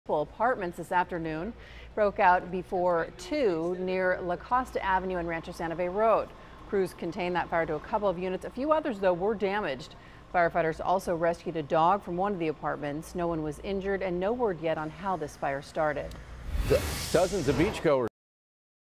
Fire Damages Several Apartments
A two-alarm fire Saturday damaged multiple apartments and firefighters rescued a dog, fire officials said.